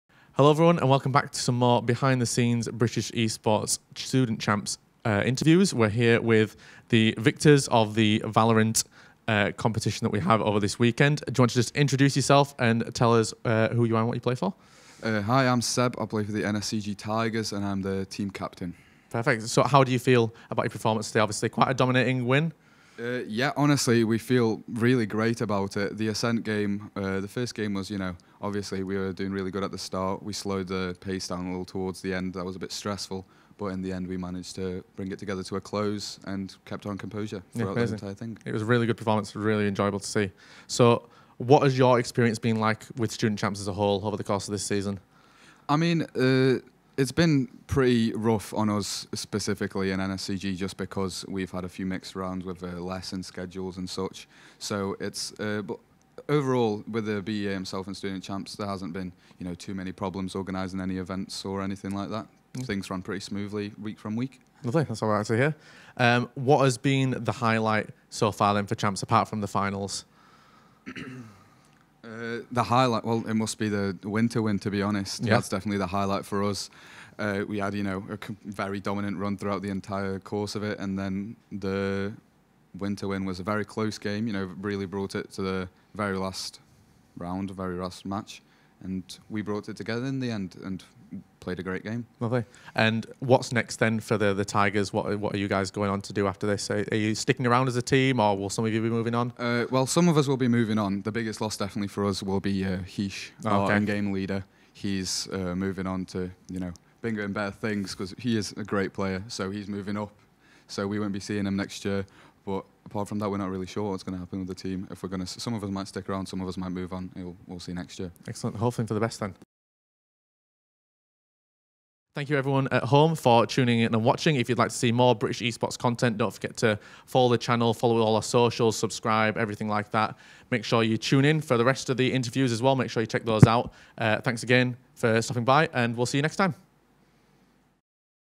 Benseba1 // Champs Finals Interview
Join us as we speak to Benseba1 from NSCG Tigers VAL about the British Esports Student Champs, the finals, and more!